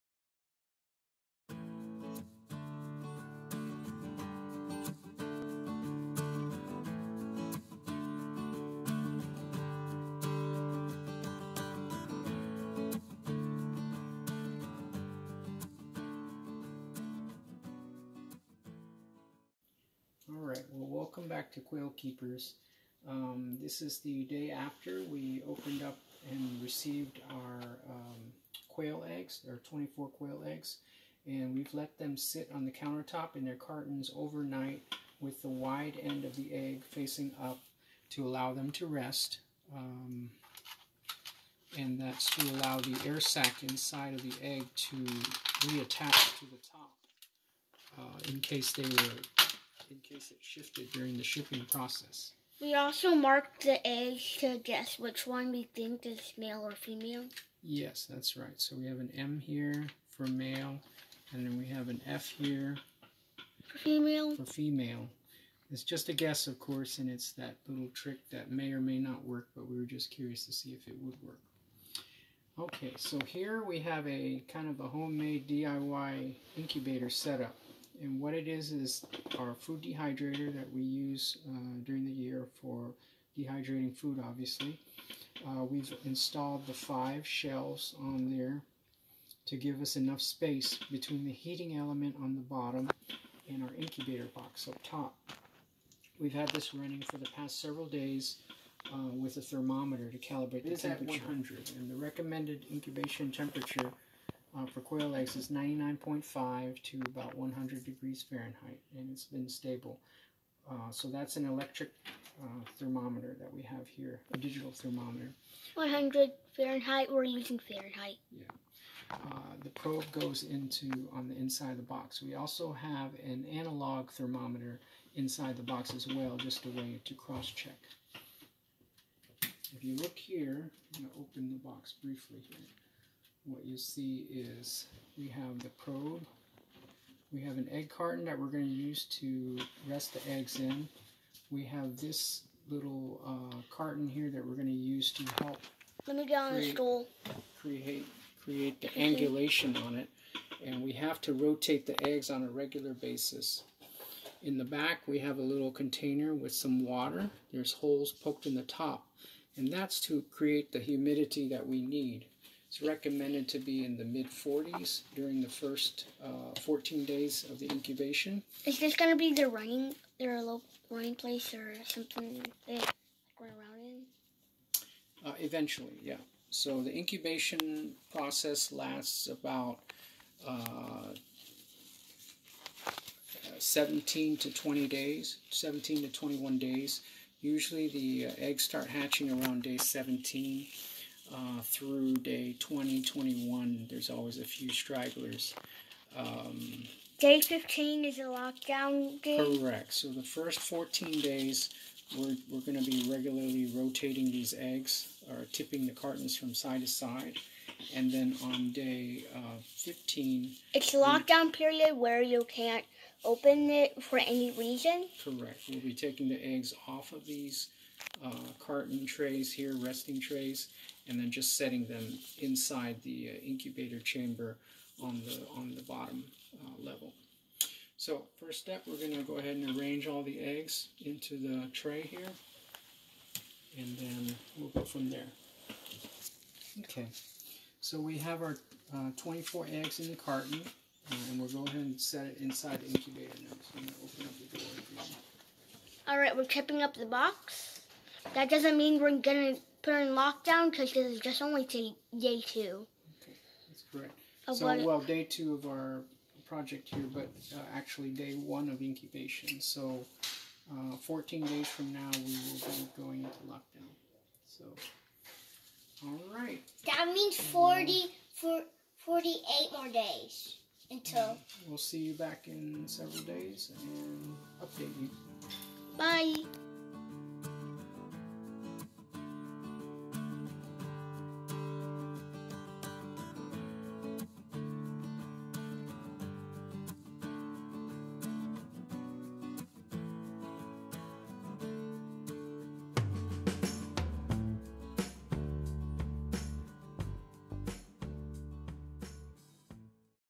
QuailKeepers Ep 2 - Starting Quail Egg Incubation in Homemade Incubator
Watch as we start incubating our quail eggs in our homemade incubator.  Will any eggs hatch?